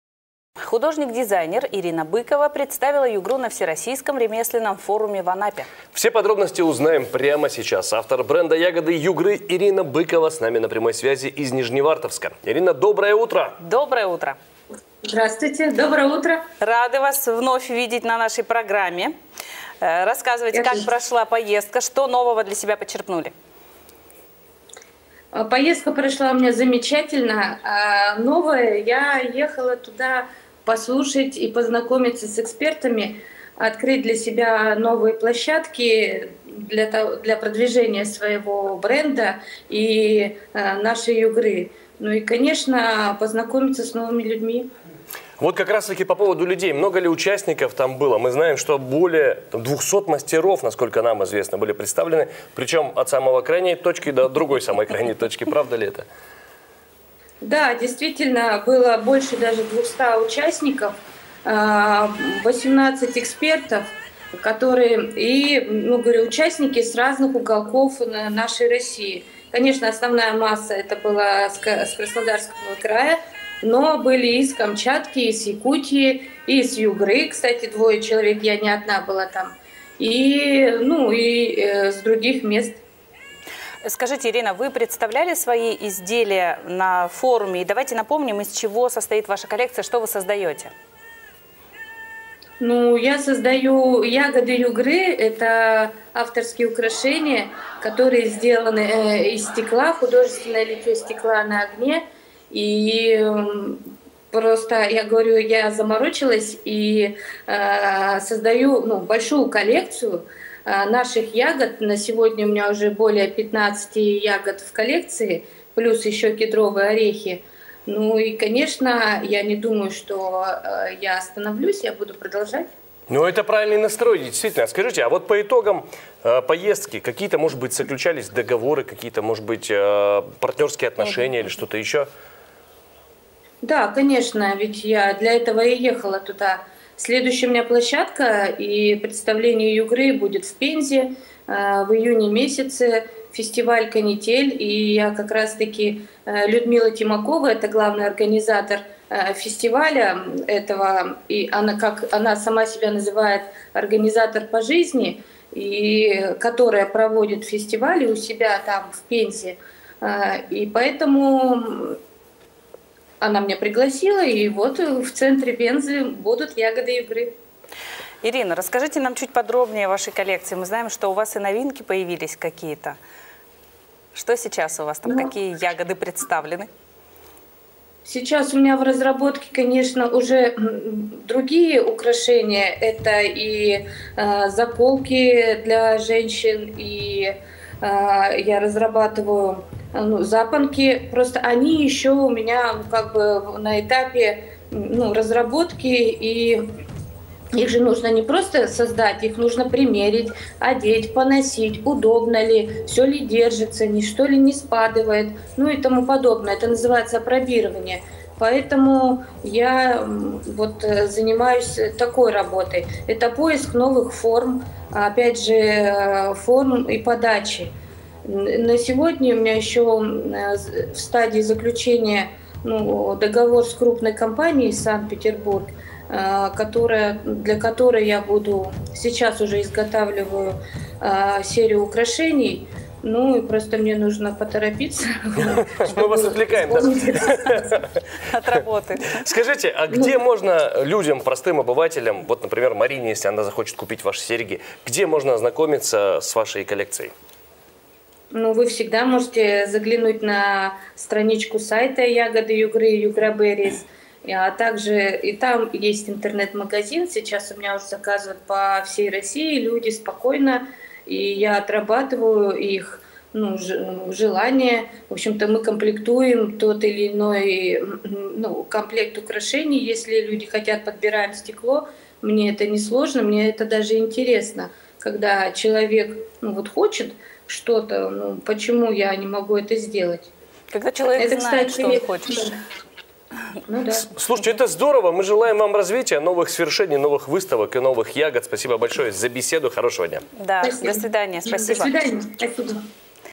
Югорчанка представила округ на Всероссийском ремесленном форуме в Анапе
На прямой связи Ирина Быкова, художник по стеклу, создатель бренда «Ягоды Югры».